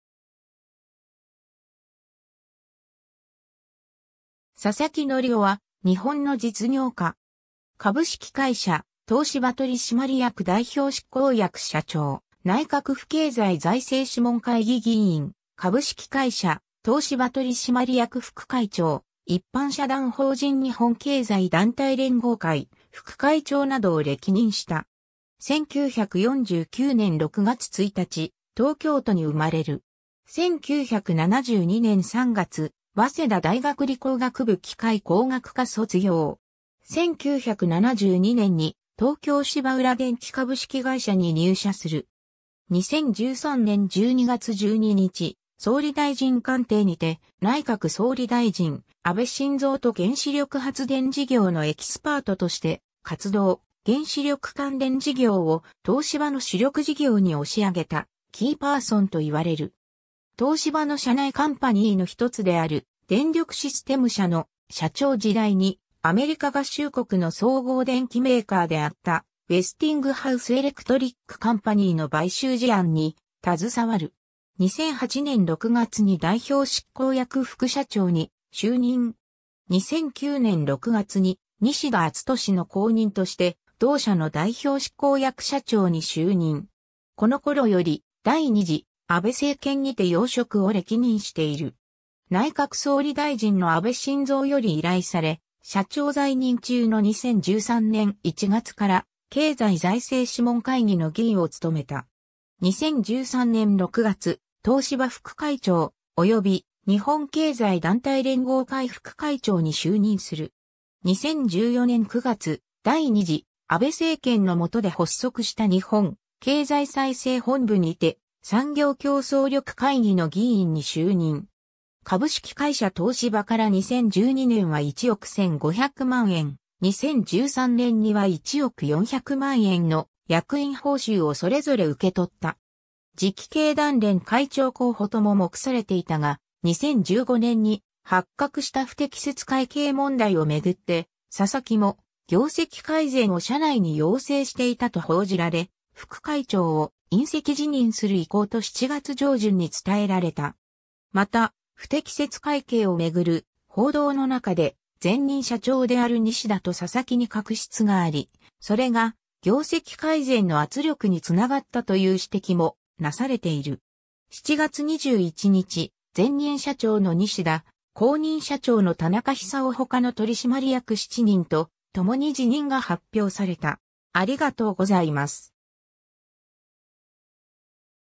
佐々木則夫 (実業家)
佐々木 則夫（ささき のりお、1949年6月1日 - ）は、日本の実業家。
 株式会社東芝取締役代表執行役社長、内閣府経済財政諮問会議議員、株式会社東芝取締役副会長、一般社団法人日本経済団体連合会副会長などを歴任した。
 1949年6月1日、東京都に生まれる。
1972年3月、早稲田大学理工学部機械工学科卒業。
1972年に東京芝浦電気株式会社（現株式会社東芝）に入社する。
 2013年12月12日、総理大臣官邸にて内閣総理大臣安倍晋三（右）と 原子力発電事業のエキスパートとして活動、原子力関連事業を東芝の主力事業に押し上げたキーパーソンと言われる。
東芝の社内カンパニーのひとつである「電力システム社」の「社長」時代に、アメリカ合衆国の総合電機メーカーであったウェスティングハウス・エレクトリック・カンパニーの買収事案に携わる。
2008年6月に代表執行役副社長に就任。
 2009年6月に、西田厚聰の後任として同社の代表執行役社長に就任。
この頃より、第2次安倍政権にて要職を歴任している。
内閣総理大臣の安倍晋三より依頼され、社長在任中の2013年1月から経済財政諮問会議の議員を務めた。
2013年6月、東芝副会長、および、日本経済団体連合会副会長に就任する。
2014年9月、第2次安倍政権の下で発足した日本経済再生本部にて、産業競争力会議の議員に就任。
株式会社東芝から2012年は1億1500万円、2013年には1億400万円の役員報酬をそれぞれ受け取った。
 次期経団連会長候補とも目されていたが、2015年に発覚した不適切会計問題をめぐって佐々木も業績改善を社内に要請していたと報じられ、副会長を引責辞任する意向と7月上旬に伝えられた。
また、不適切会計をめぐる報道の中で、前任社長である西田と佐々木に確執があり、それが業績改善の圧力につながったという指摘もなされている。
7月21日、前任社長の西田、後任社長の田中久雄他の取締役7人とともに辞任が発表された。